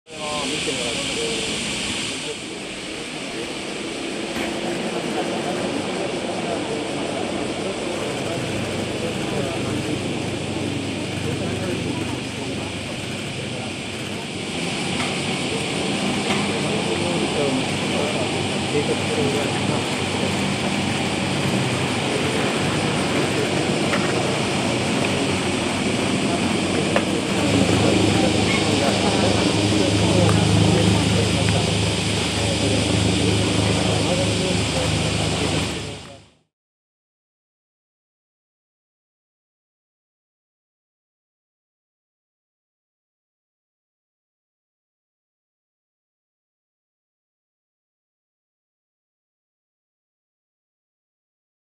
安倍元首相銃撃事件 鑑定留置のために移送される山上容疑者
安倍晋三元首相（67）が奈良市で参院選の街頭演説中に、銃で撃たれて殺害された事件で、奈良地検は25日、無職の山上徹也容疑者（41）【殺人容疑で送検】の精神状態を調べるため、鑑定留置を始める。